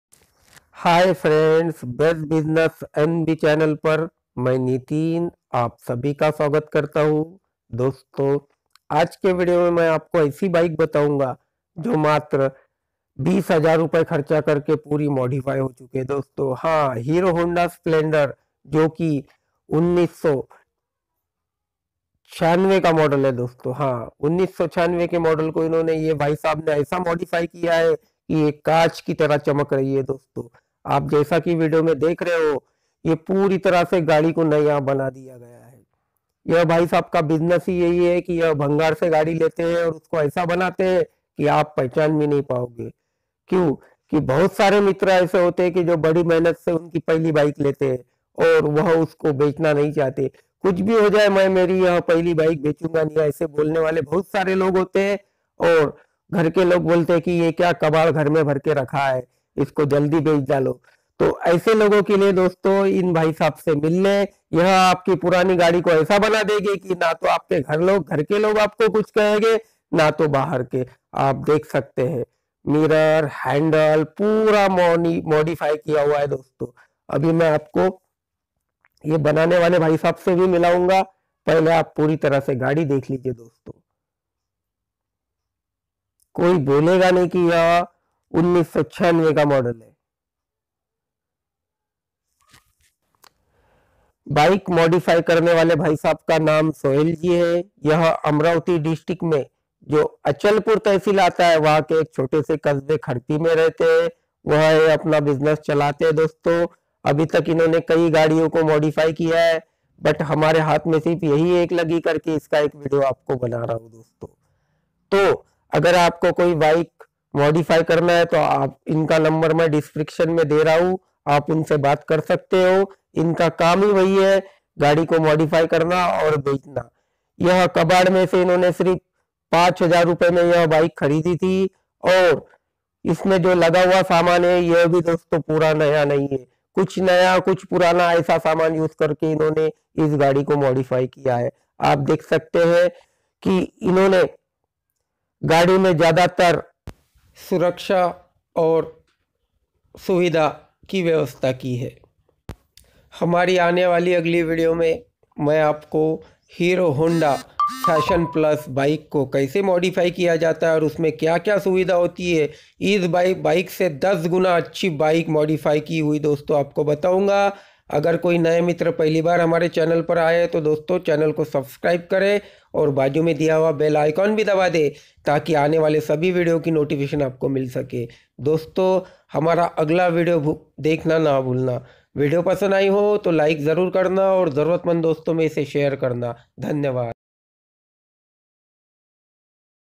bike modified splendor | hero honda Splendor modified
hero honda Splendor baik , बाइक ko कैसे modified मोडीफाय किया है और इसमे कितना खर्च आया ये सभी बातें आपको देखने मिलेगी

अगर आपका कोई प्रश्न है तो आप मुझे कमेंट कर सकते है.

अधिक जानकारी के लिए चॅनल को subscribes करे
धन्यवाद